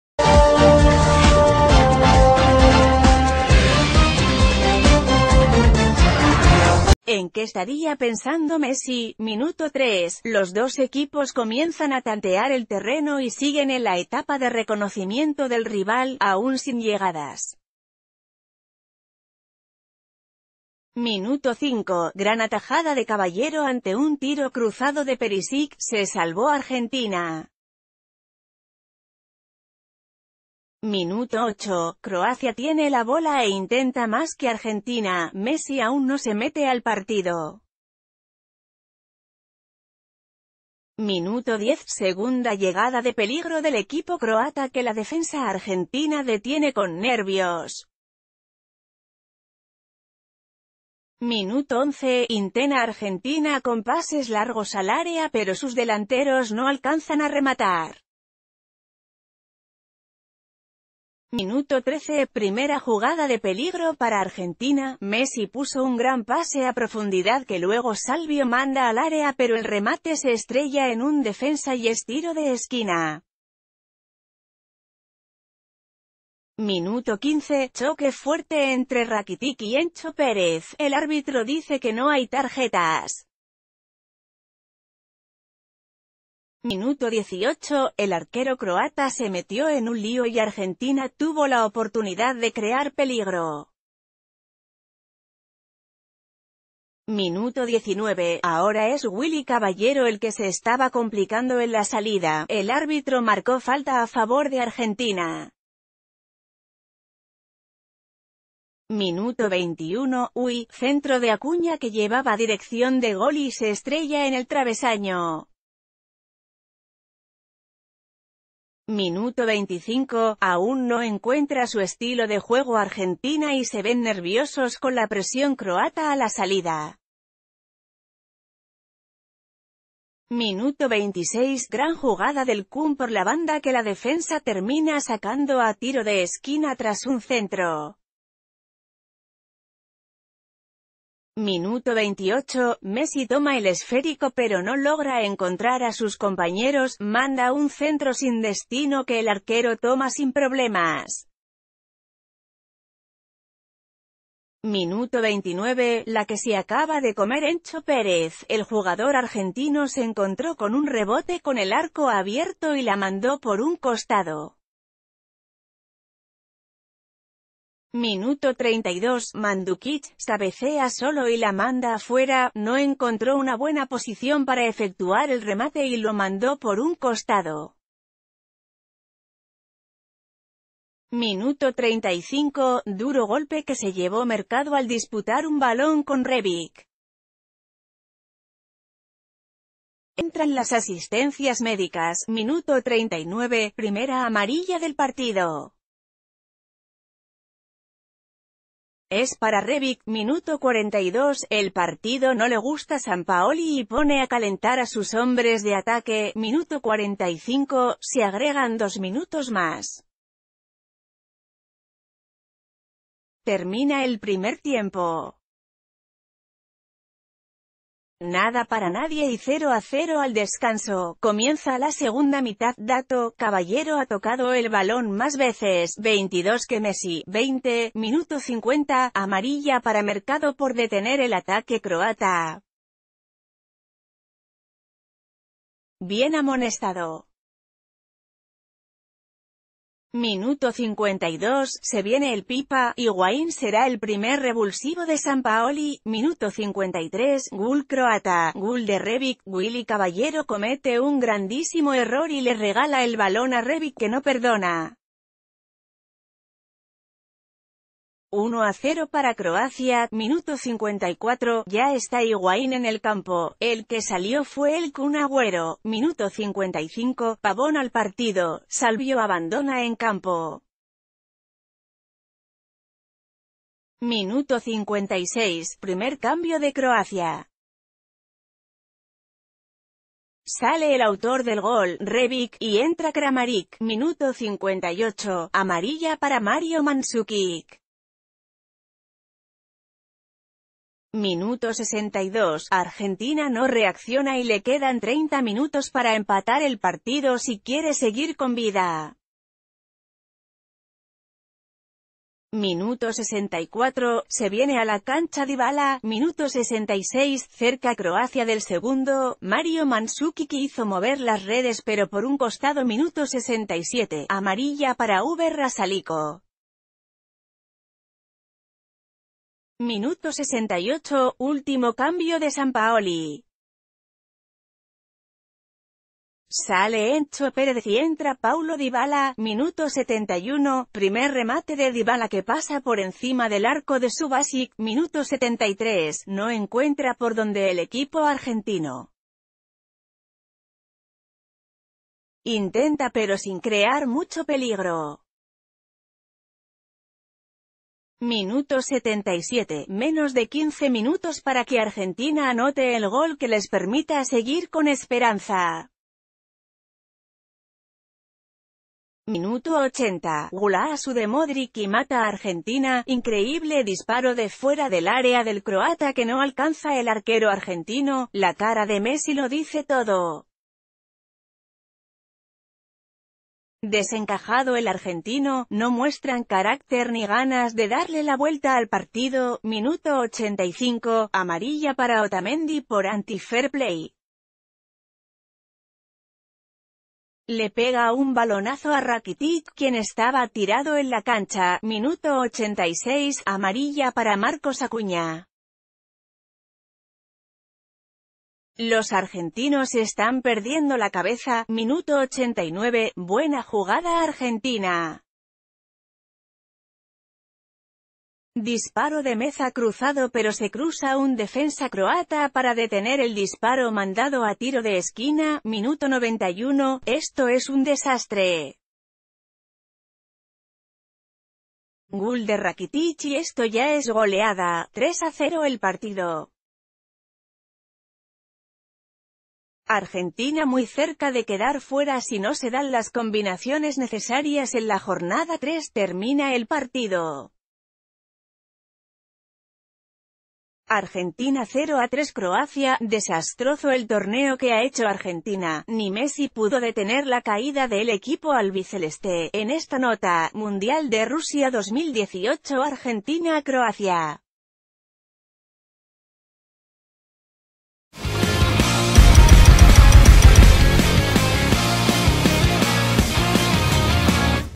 ¡Sigue el minuto a minuto del partido Argentina vs Croacia!
¿En qué estaría pensando Messi. Minuto 3: Los dos equipos comienzan a tantear el terreno y siguen en la etapa de reconocimiento del rival. Aún sin llegadas. Minuto 5: ¡Gran atajada de Caballero ante un tiro cruzado de Perisic! Se salvó Argentina. Minuto 8: Croacia tiene la bola e intenta más que Arg...